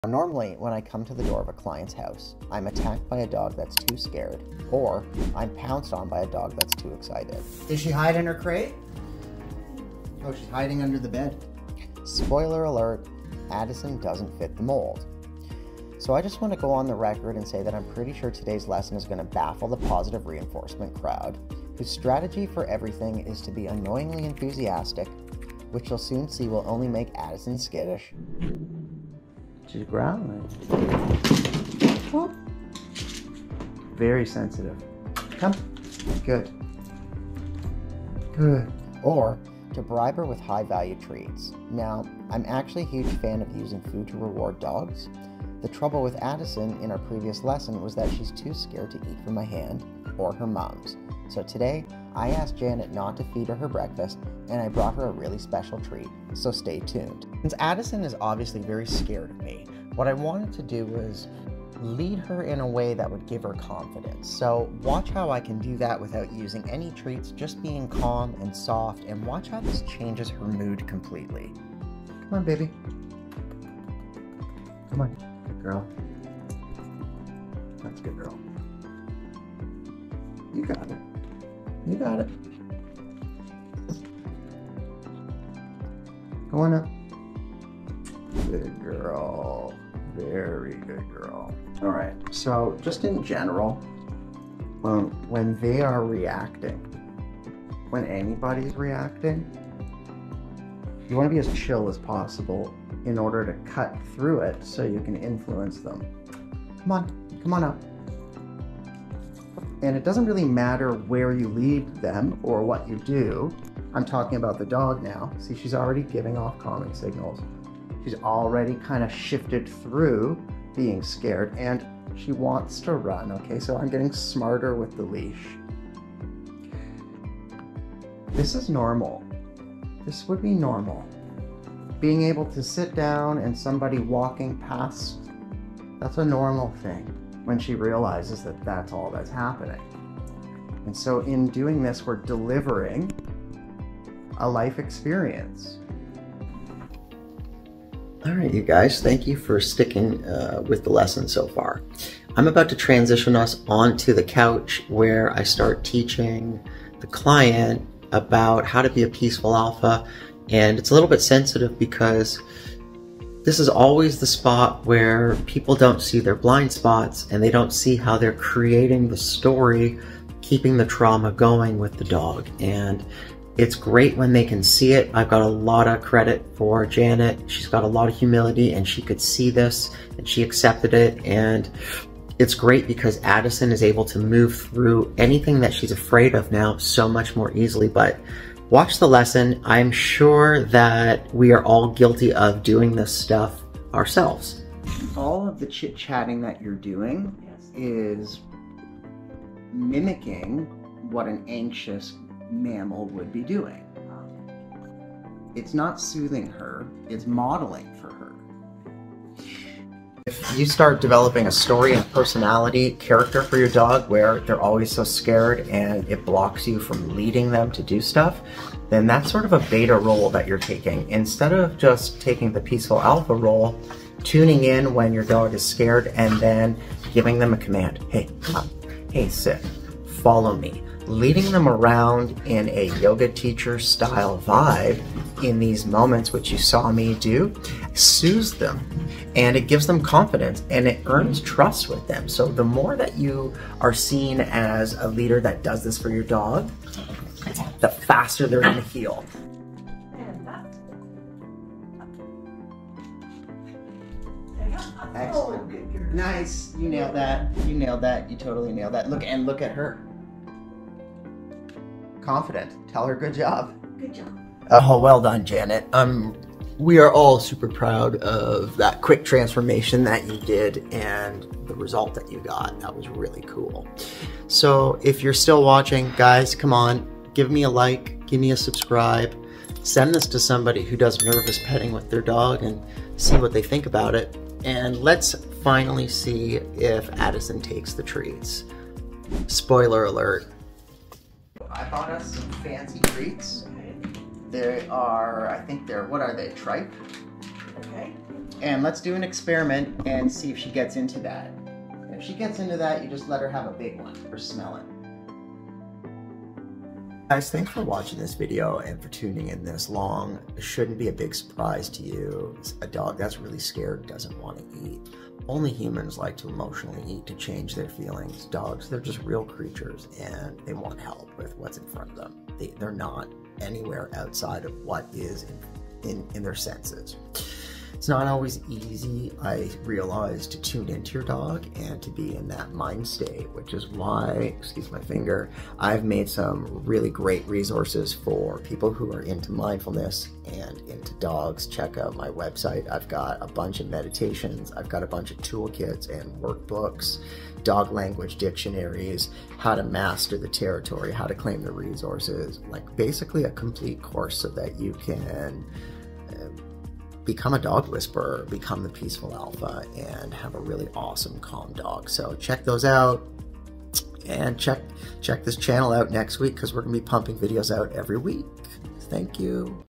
How to be the Peaceful Alpha to a Traumatized Rescue
Today I'm training Addison.  She is a dog that refuses high value treats because she has a lot of fear and trauma from her past life where she grew up on a puppy farm.

You have to use common sense guys.  Popular culture wants you to feel that positive reinforcement, high value treats, and enthusiasm can fix everything.  It can't.  What can fix everything is LOVE.

So please tune in to today's lesson and watch how a Peaceful Alpha teaches and leads using LOVE and it's different flavors.  Today it's patience, kindness, calmness, encouragement, being present, wisdom and compassion.  These are the virtues of a Peaceful Alpha, and this is what your dog needs you to be in order to respect you and feel safe.

If you liked this lesson, I have a ton of cool resources you might also like.